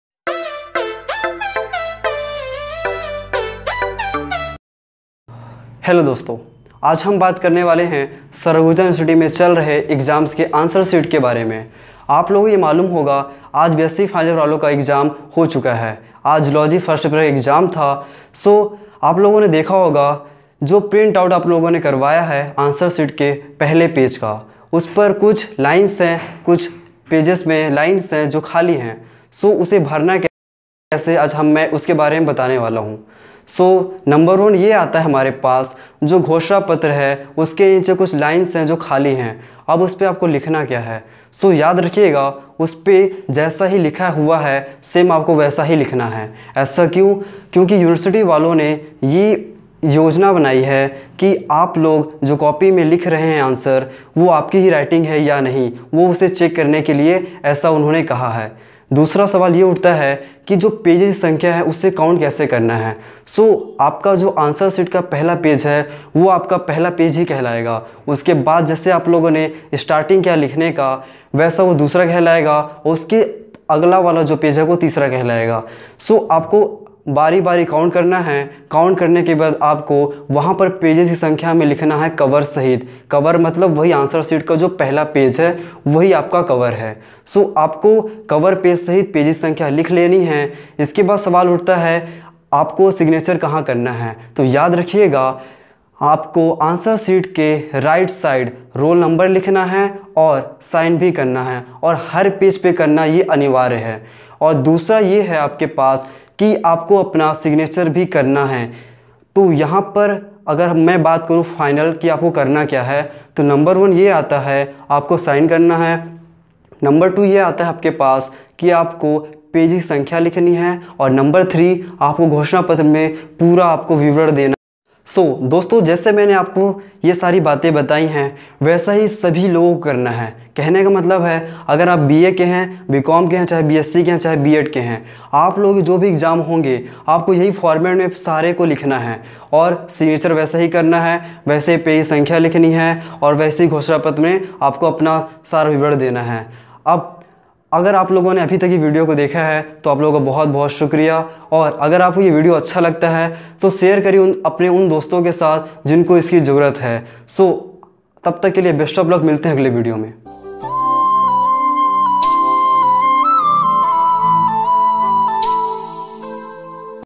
Sant gahira guru university l उत्तर पुस्तिका, घोषणा पत्र में क्या लिखना है l pages की संख्या कैसे l
Sant gahira guru university l उत्तर पुस्तिका में घोषणा पत्र में क्या लिखना है, pages की संख्या कैसे लिखनी है और signature कैसे करना है
Full details in Hindi by Irshad Ali

I hope ye information काम आए आप लोगों के लिए